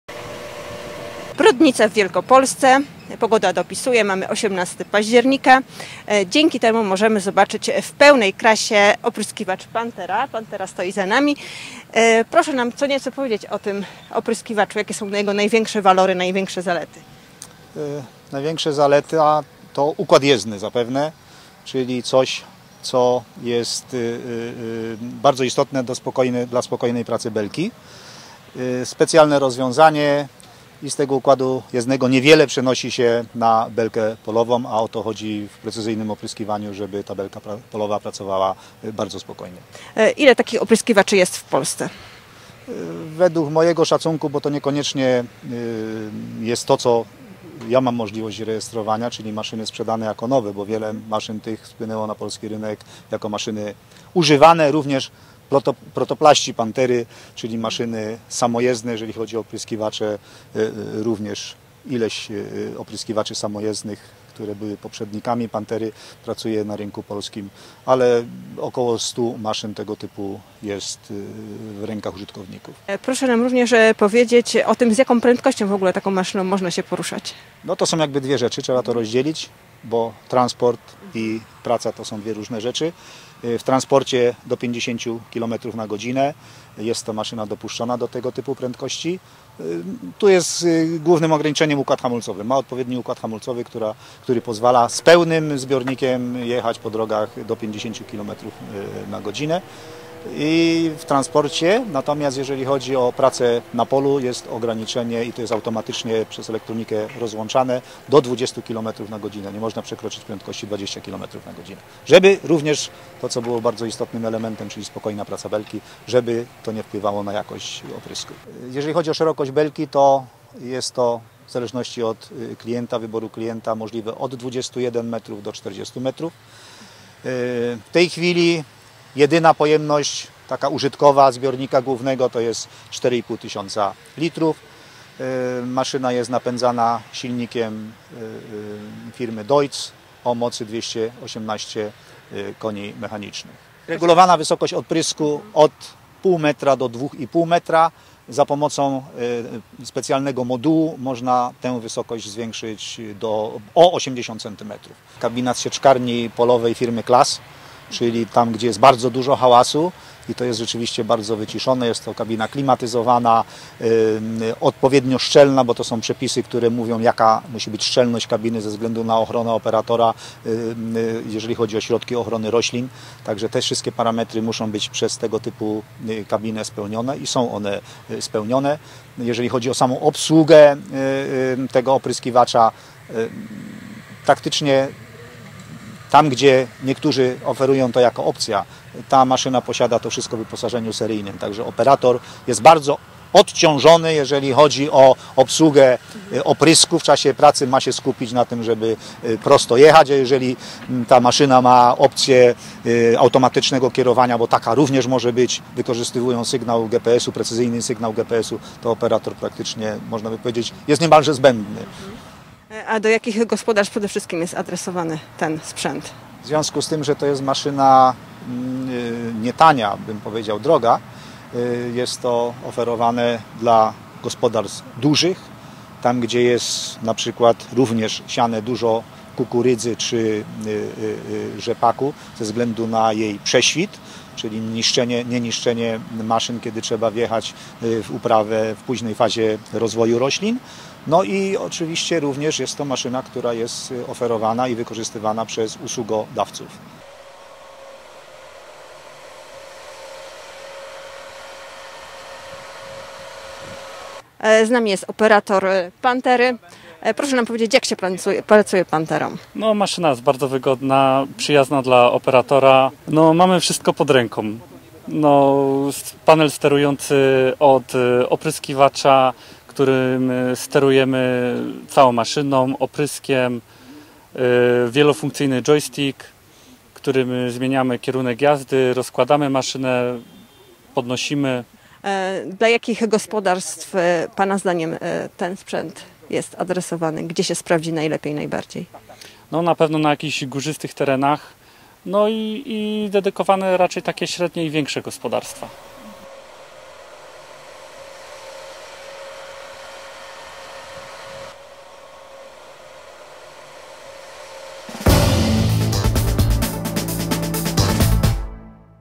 Opryskiwacz Amazone na polu w akcji. Nawet 40 m szeroki.
Zobacz Panterę w akcji. Jaki ma zbiornik? Do jakich gospodarstw się nadaje?